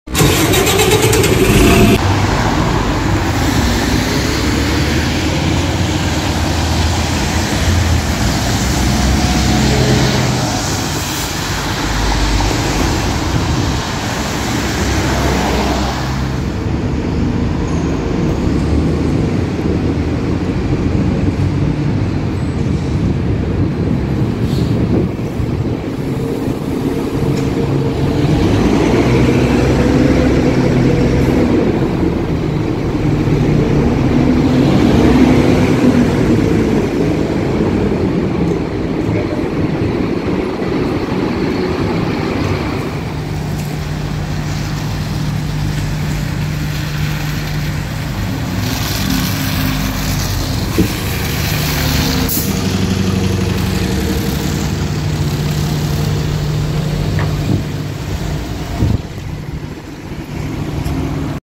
Farewell to Arriva Aylesbury EP2 | Aylesbury’s Enviro 400s
Join me for my series of videos released on Saturday 27th July 2024, though out the day.

The Aylesbury and High Wycombe depots of Arriva will be closing today due to financial reasons.

In light of this, I have put together several videos that I have recorded over the past few years.